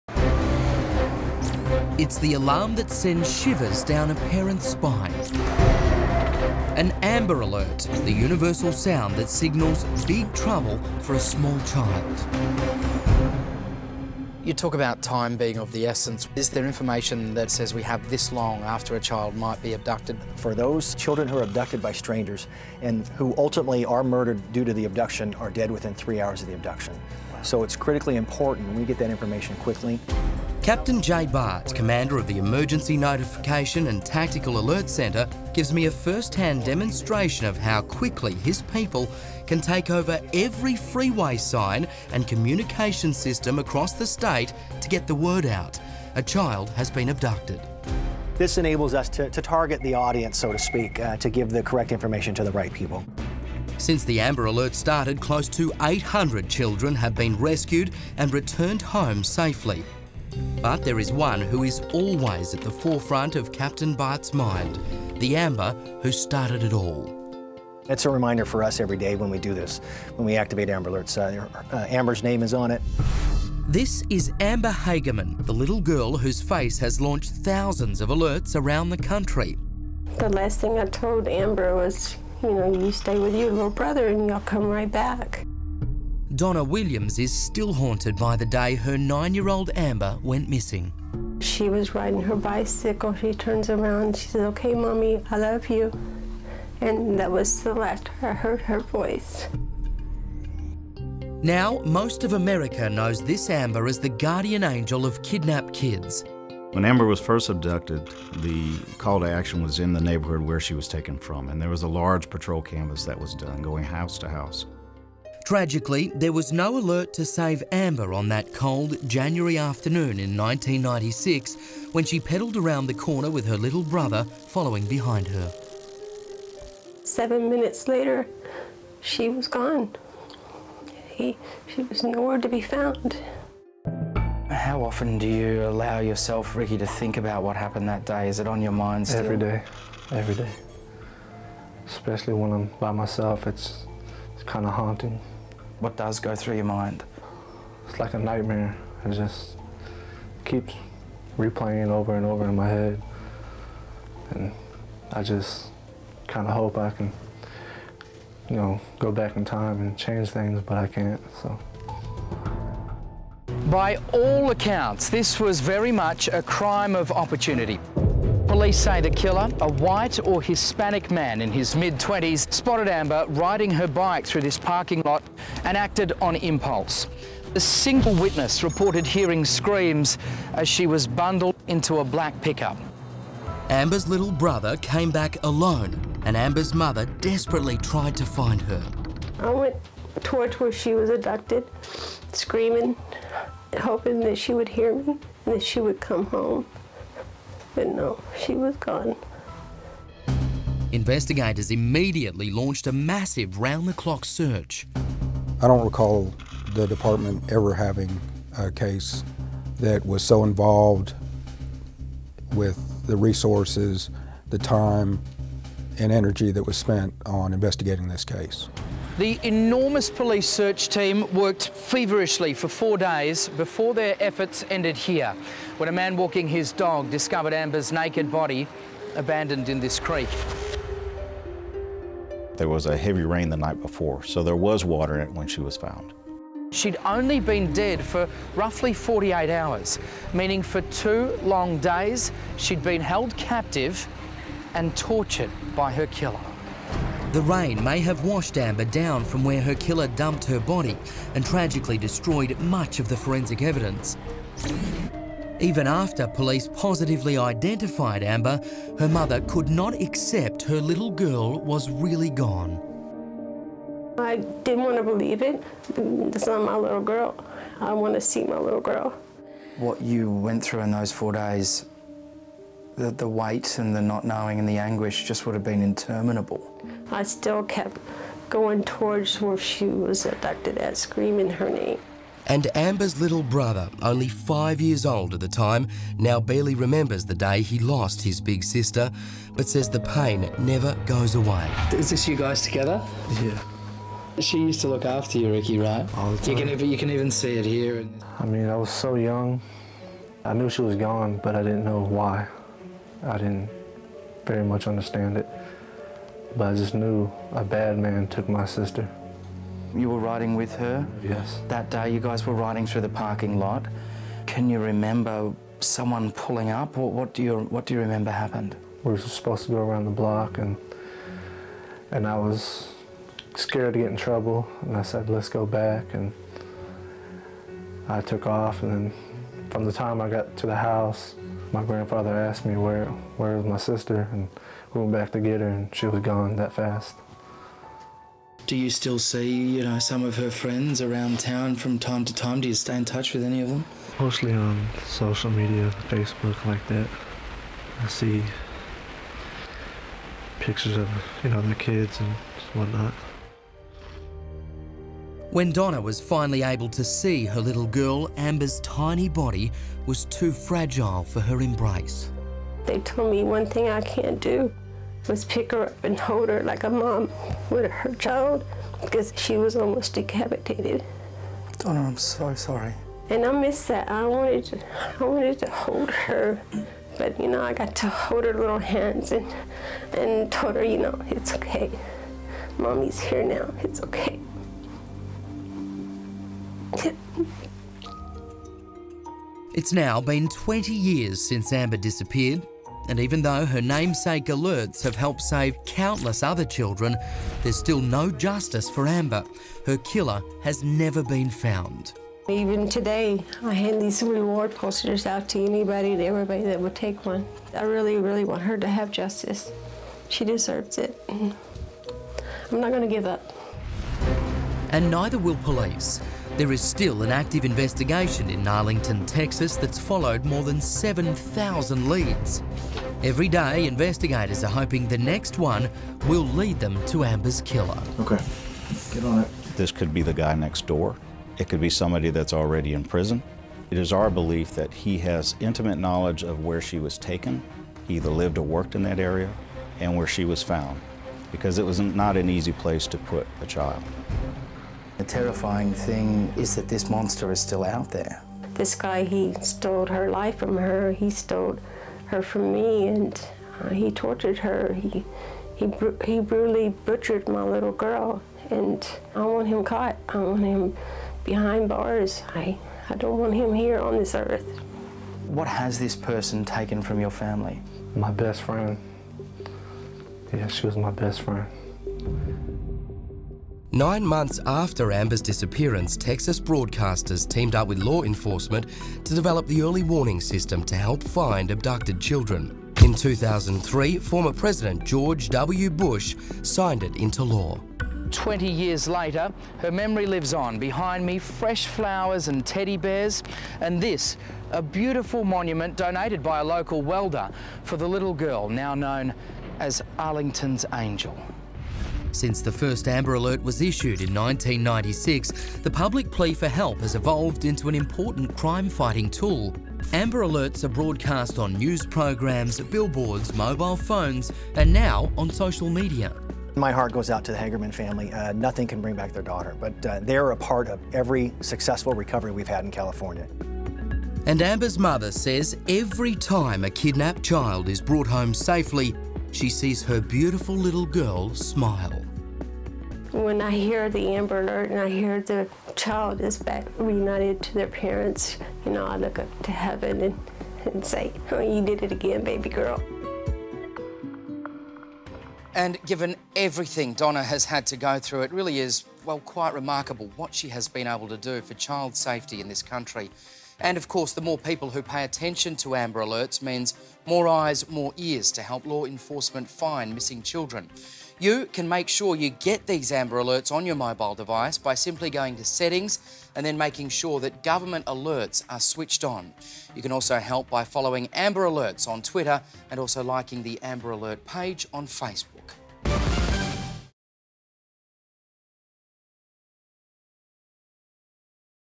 'I Want Her to Have Justice': The story that inspired the AMBER Alert system
Amber Hagerman's disappearance helped to launch the program that went on to save hundreds of abducted children. Her mother talks about the fight. More